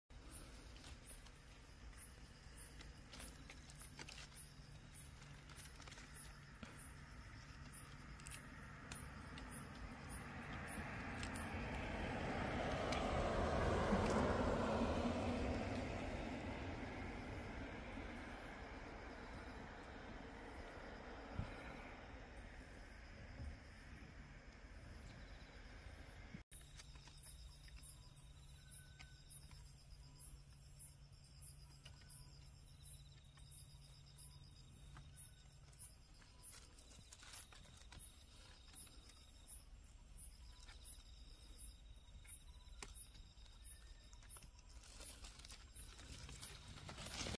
한반도 최강의 풀벌레를 건드려보았습니다
한반도 최강의 곤충 여치베짱이!
건드리면 어떻게 될까요?

Giant eulalia conehead (Parvarhynchus japonicus)